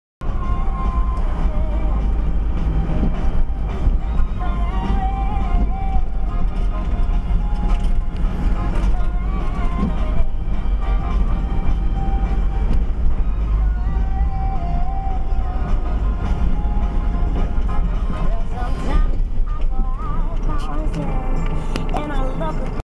Van driver awareness, kid on a bike (HD)
A young lad on a bicycle on the pavement is quite clearly not in full control. The guy driving the small van is aware of this and is ready to hit the brakes, which (fortunately for the kid) he does.